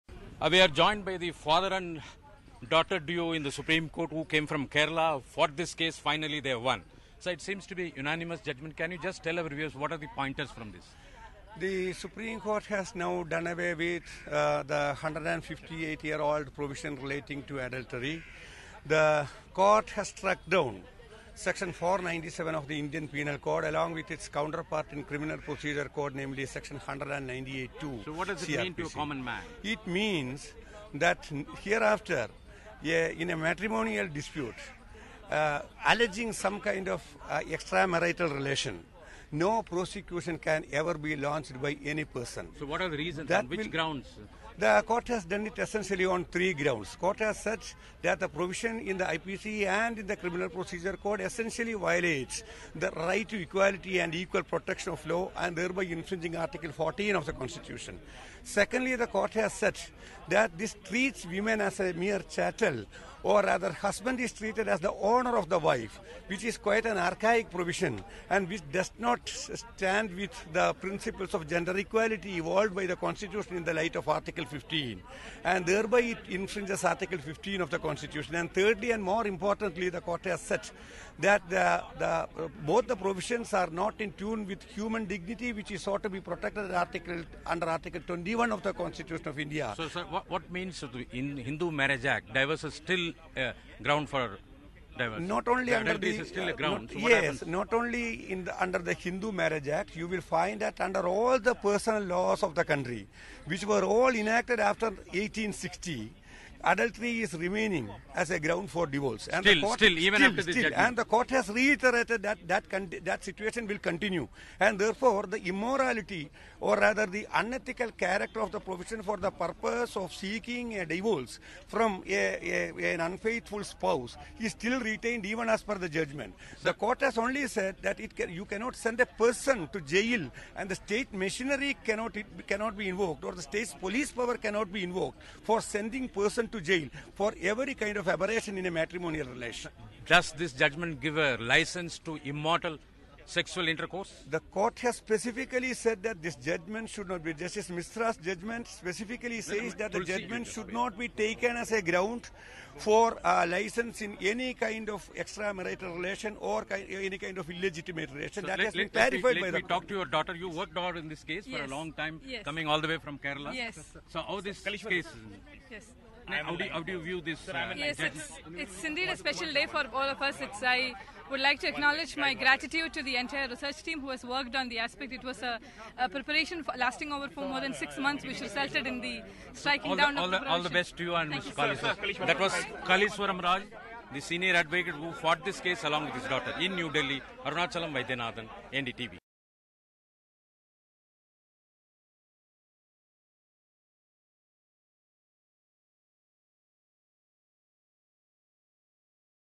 Adultery Not A Crime, Law Is Unconstitutional, Rules Supreme Court
Adultery is no longer a crime in India even though, "without a shadow of doubt", it can be grounds for divorce, the Supreme Court said today, scrapping a 158-year law that punished a man for an affair but not the woman, treating her as "property". "A man having sexual intercourse with a married woman is not a crime," said Chief Justice Dipak Misra, calling the Victorian adultery law arbitrary.